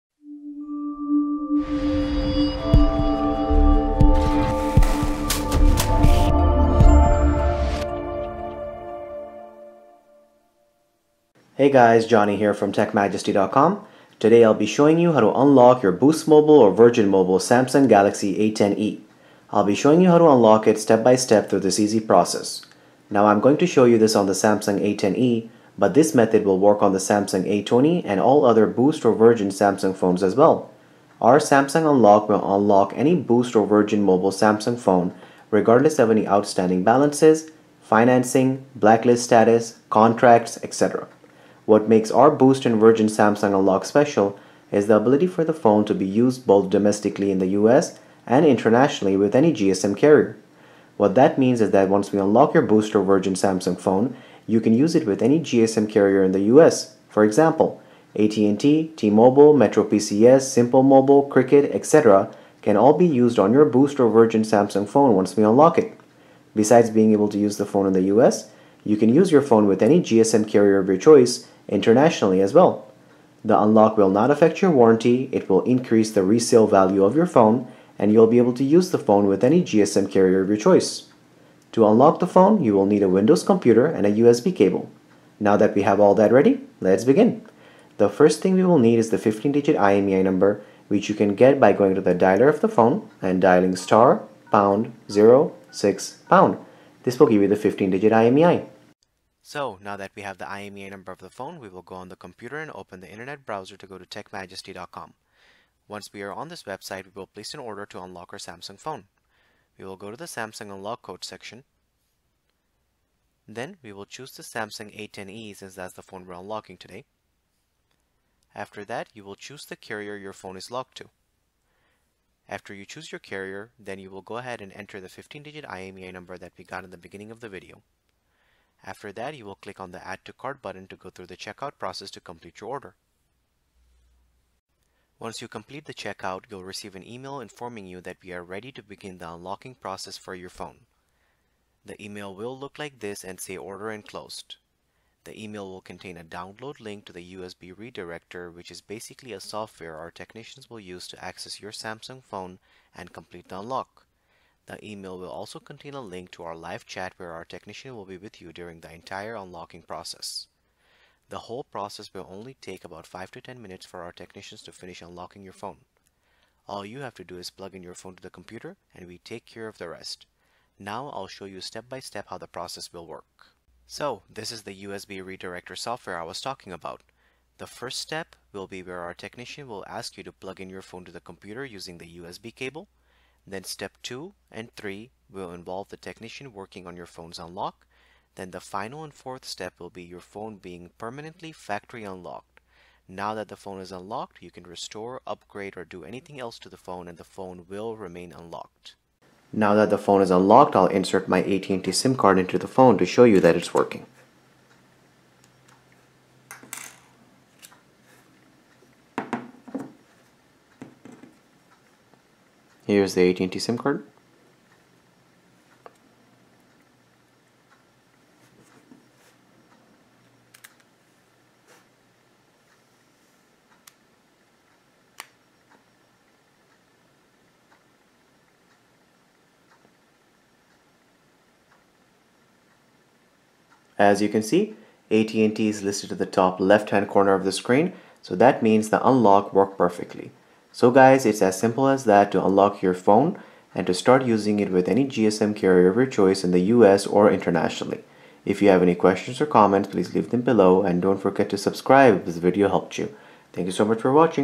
How to Unlock Virgin Mobile & Boost Mobile Samsung Galaxy A10e & A20 - Use in USA & Worldwide
- How to Unlock Boost Mobile Samsung Galaxy A10e and Unlock Boost Mobile Samsung Galaxy A20!

How to Unlock Virgin Mobile Samsung Galaxy A10e and Unlock Virgin Mobile Samsung Galaxy A20!

Unlock Virgin/Boost Samsung Galaxy A10e and A20 in a few simple steps to use both domestically in the US and internationally with any GSM carrier of your choice!

Once we unlock your Virgin/Boost Samsung A10e and A20 you can use it with any GSM carrier in the US such as AT&T, T-Mobile, Metro PCS, Simple Mobile, H2O Wireless, Cricket, Straight Talk, etc.

Fast and easy unlock. No technical knowledge is required!